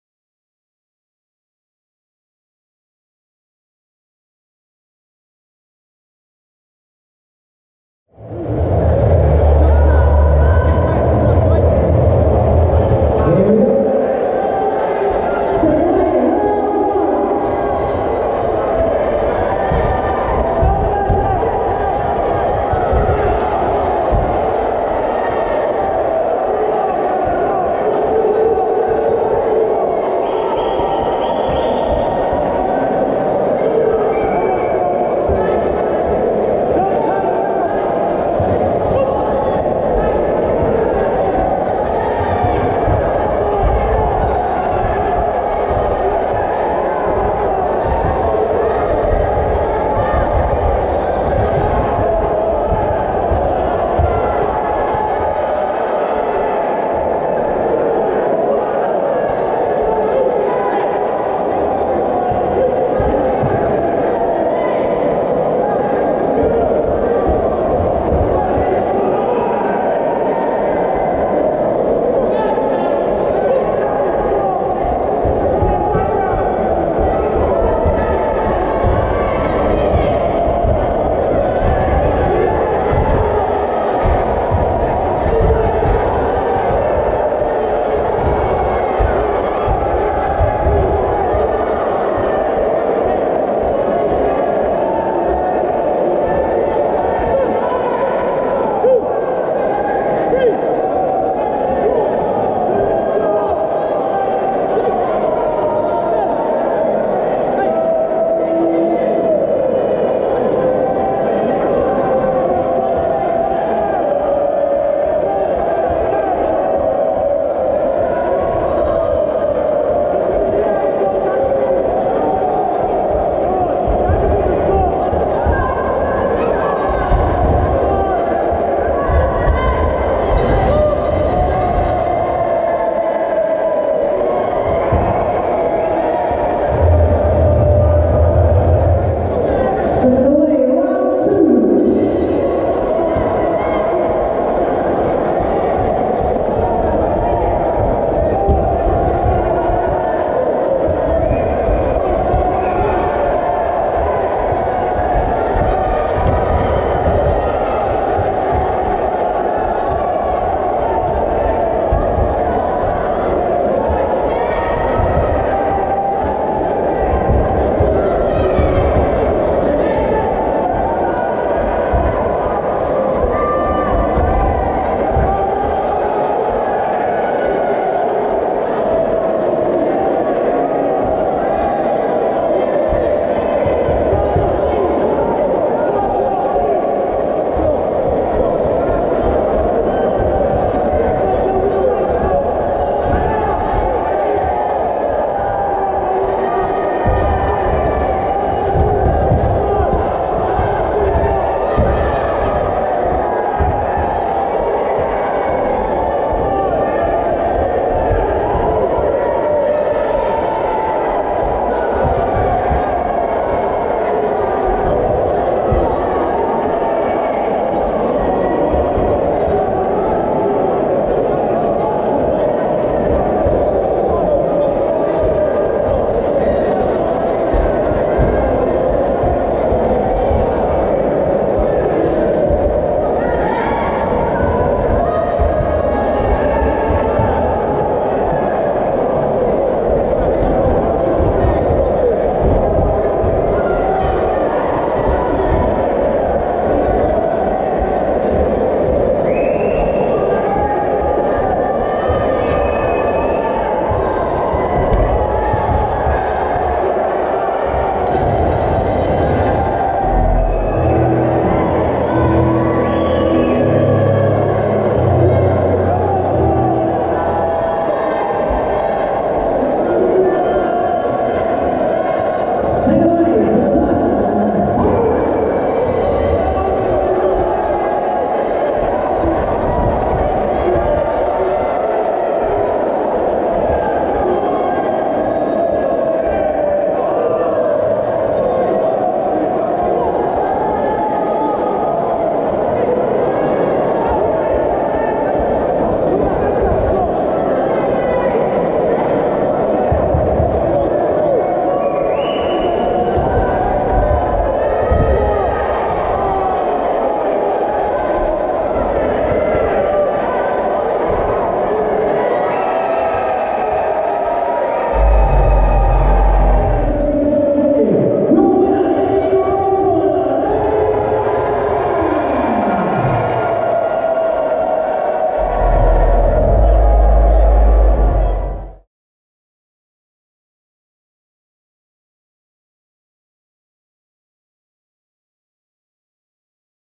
Ultra White Collar Boxing | Romford | Danny McNab VS Scottie Day
Romford Ultra White Collar Boxing 7th July 2018

8 Weeks FREE training / Get in the best shape of your life/ Raise money for Cancer Research UK